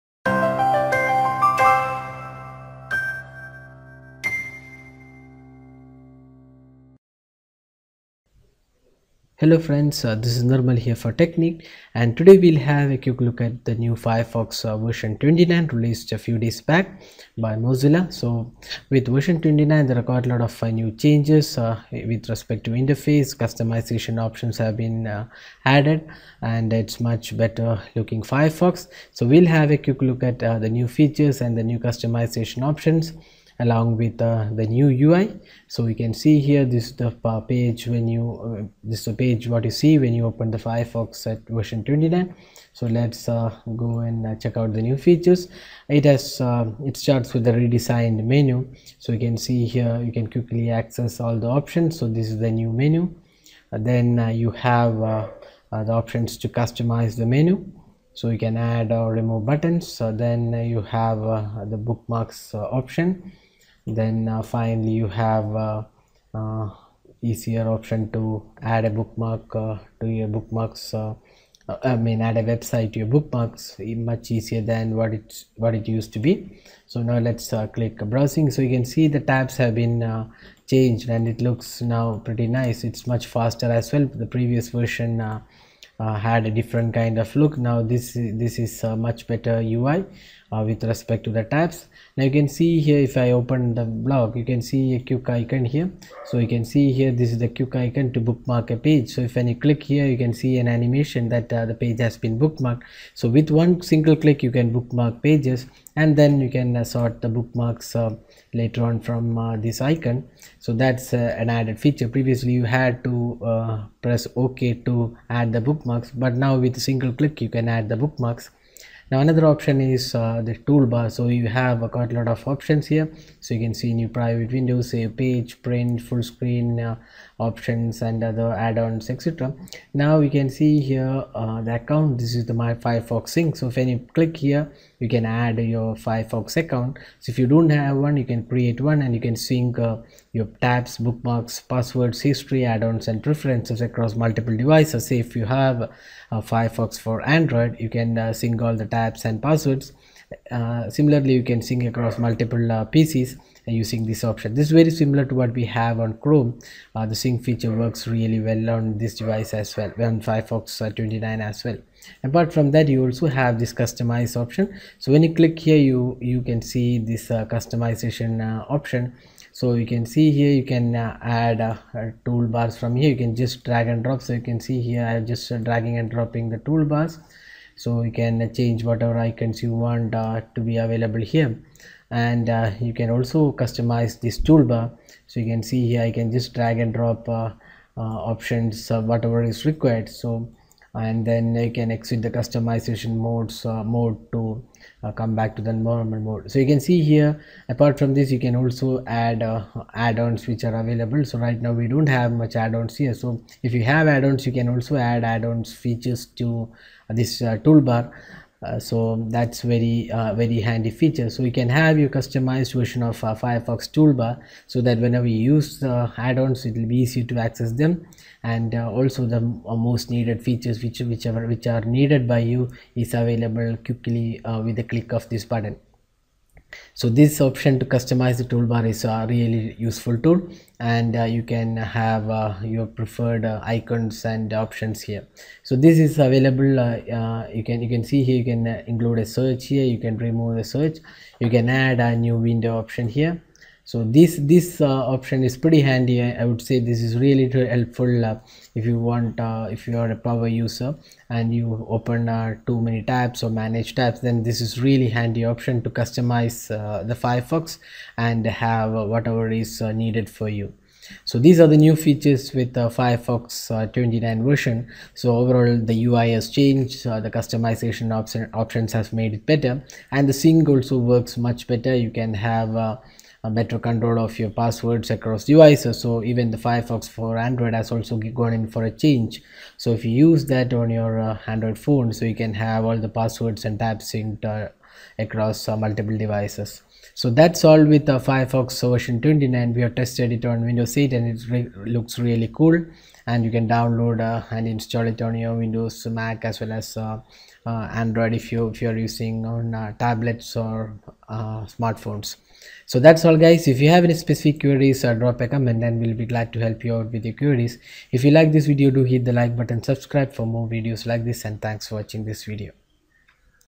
Firefox 29 for Windows- Review
Mozilla has released the latest version of Firefox 29 with new tab design, customization options and also better features. We take a look at the new features of Firefox 29.